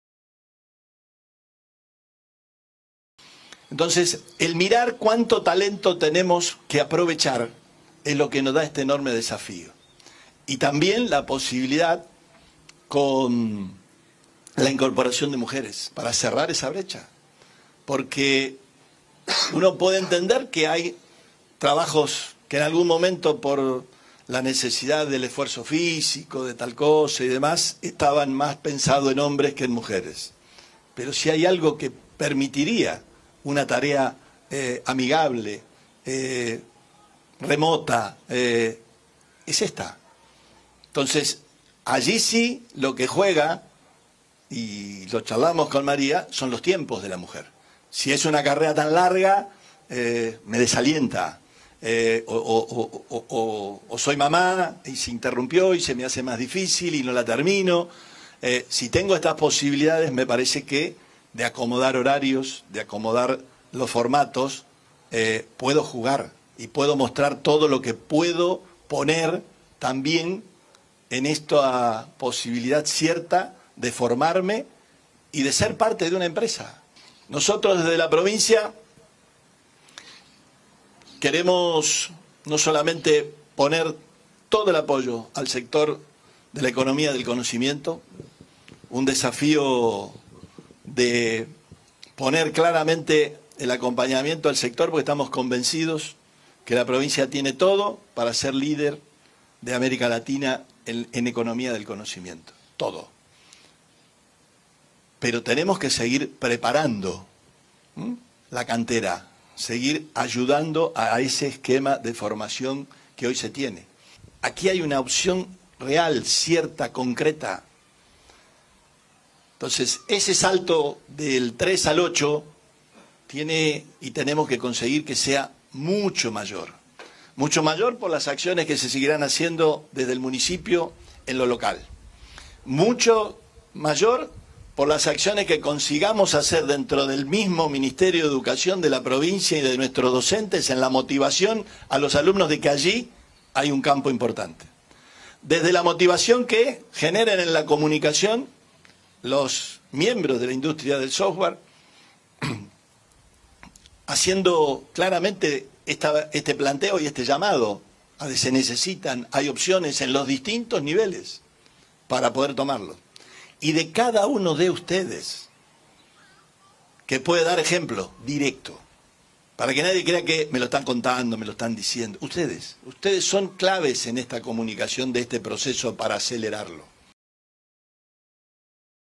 PEROTTI: “SANTA FE TIENE EL POTENCIAL PARA SER LÍDER DE AMÉRICA LATINA EN ECONOMÍA DEL CONOCIMIENTO”
El gobernador participó del inicio del segundo nivel de “Argentina Programa” en Rafaela, una estrategia de formación para el trabajo que se complementa con el programa local “Elegí Digital”.